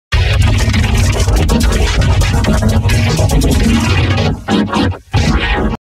(COOL BERRY JUICE POWER VIDEOUP V2 IDEA) An Audio Effect I'll Call "BJP/VUV2 Combo"
(aka. Klasky Csupo in BJP/VUV2 Combo)

Inspired by @MoonstoneVideoEditorCommunications296 BP/VUV2 Combo effect.

Berry Juice Power effect by @JavilenTRVM.
Videoup v2 effect by @MetallicBlizzardCoreIndustry6439 HD

How to make: Soon...